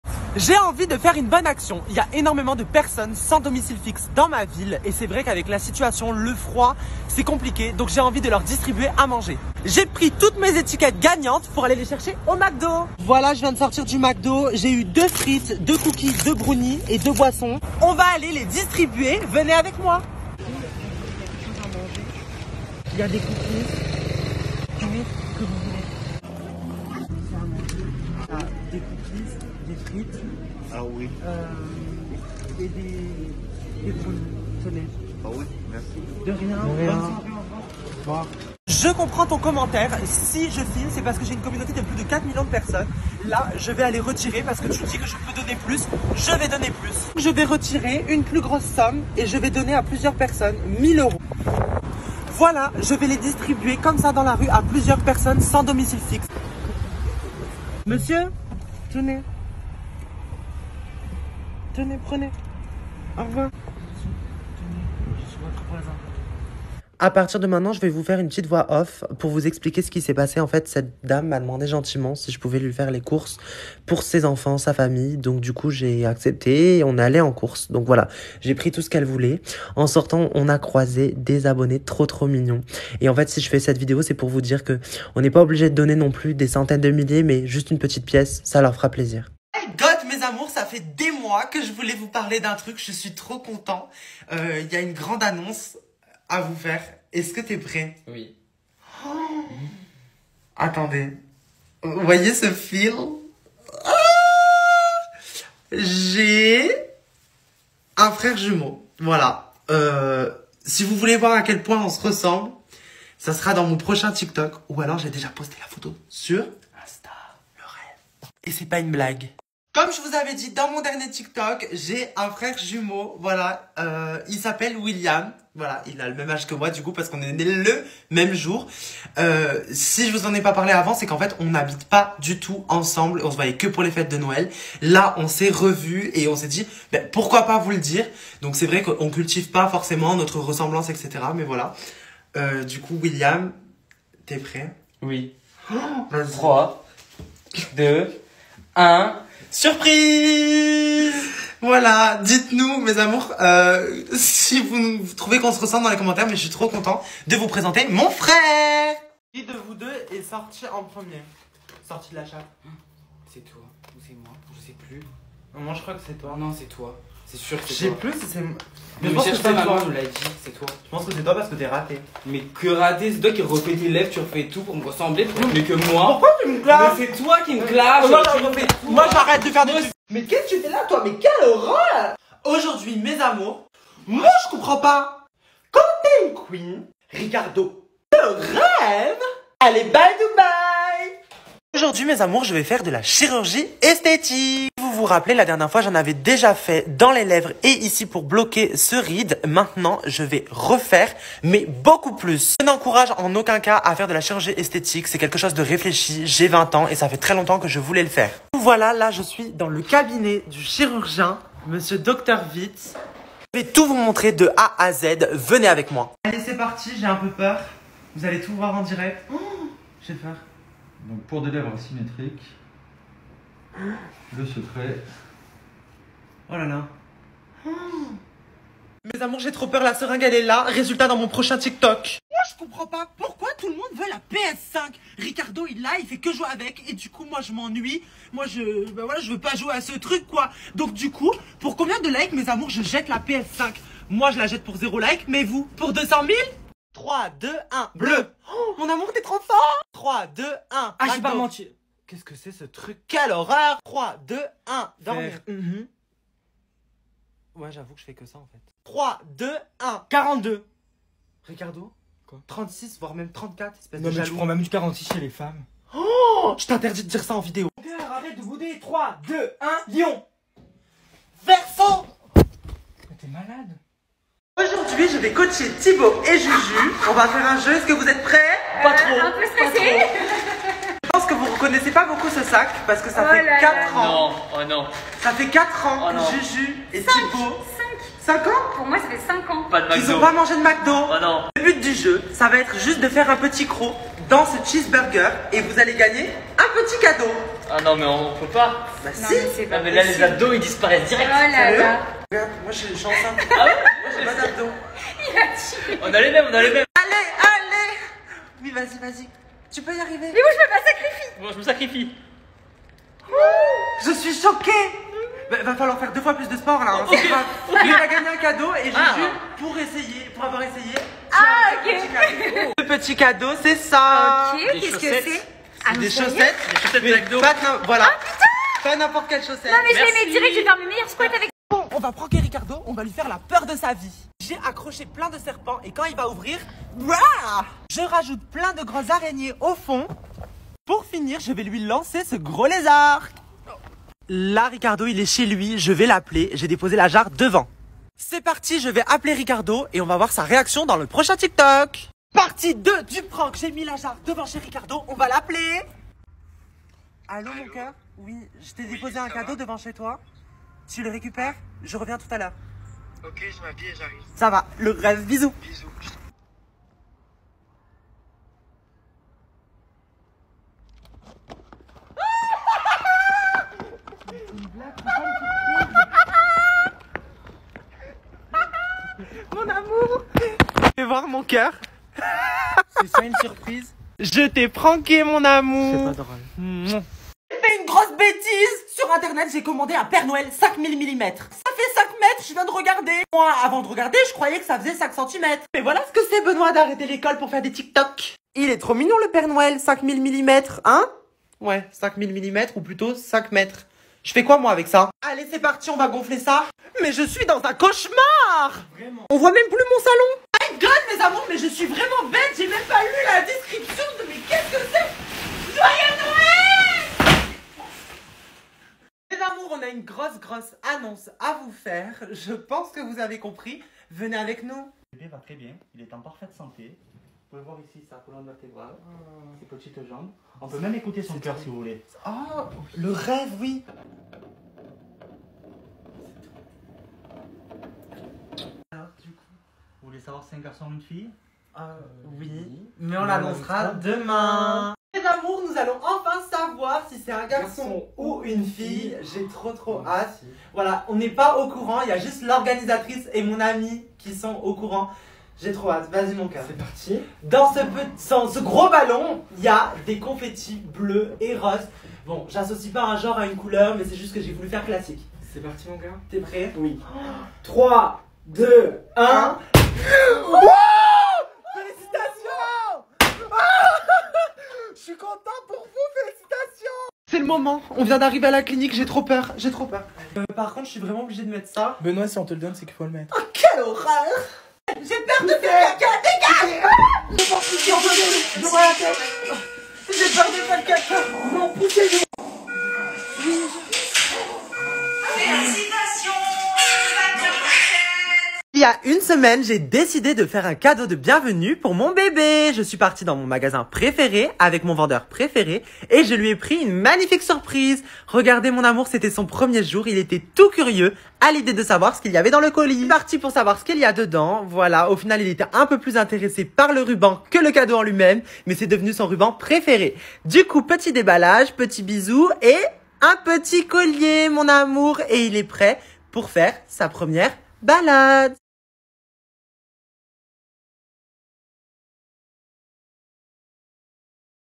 Benoît Chevalier enceinte, il accouche et vous présente son frère jumeau ! BestOf Épisode 18
Dans cette vidéo, découvrez plein de vidéos d'astuces, d'expériences plus folles que les autres, des crash-test et des avis objectifs ! Je vous dévoile ma grossesse et mon frère jumeau. Après 5 ans sans manger de Mcdo, je commande des hamburgers pour JujuFitCat et TiboInshape ! Découvrez leurs réactions ! Je ne vous en dit pas plus et vous laisse découvrir cette video ! ❤️

Crédits :
Réalisation de miniature : GROUPE OUAIS!